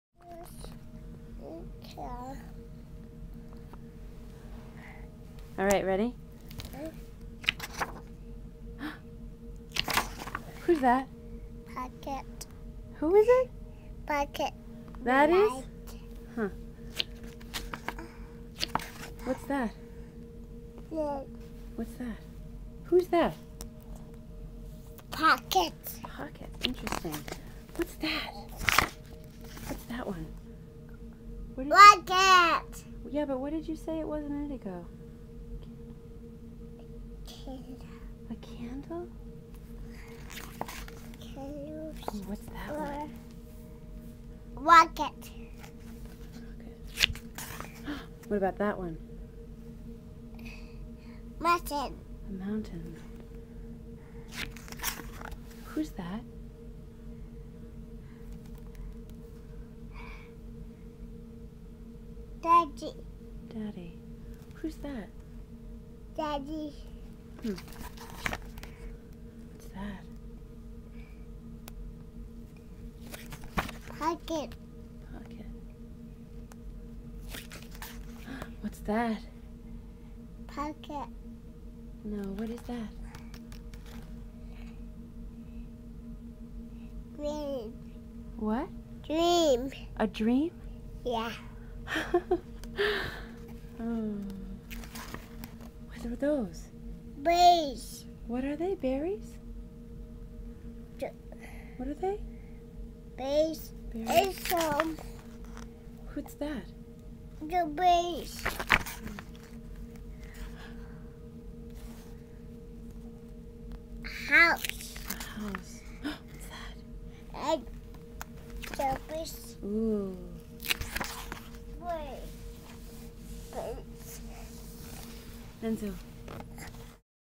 Spirit Of Art, Medium: Child, Part I
A Mother gains powerful insight into her 2 yr old's imagination and reflection of his world, when she asks him to describe what he sees in the paintings of Georgia O'Keeffe.

The boy recognizes O'Keeffe as his Mother's lover, whom he affectionately refers to as 'Pockets' (aka Paradox). He identifies rockets, candle flames, mountains, his Father in a watercolored female figure, and calls her painting (Series I, No 8) 'a dream'.

It is truly remarkable to witness the interface of the artist's spiritual attainment/expression in relationship to this child's association of world & image.